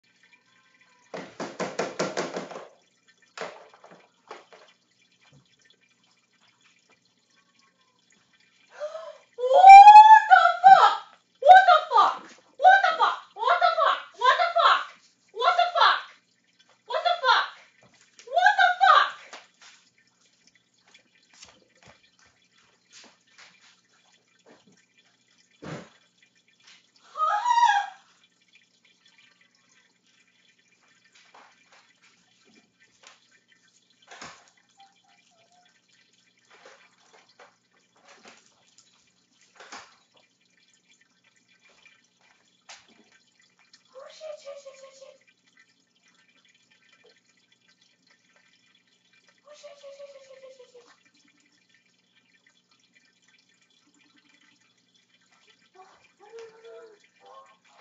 Pet owner in hysterics after cat brings live MOLE into their home
This pet owner was left in hysterics after her cat brought home a live mole and let loose.

A camera set up inside Olivia Hadley's home in Greenwood, Indiana, captured the moment 3-year-old Misser Man entered the room with a mole in his mouth.

The homeowner's pet dog is seen watching the drama unfold.

Hadley told Newsflare: "My cat Misser Man brought an alive mole in through my living room window while I was cleaning which caught me off guard.

"I LOVE all creatures so once I saw what it was and got the cat put away I got the mole and took him back outside and kept my cat in so the mole could have time to hide."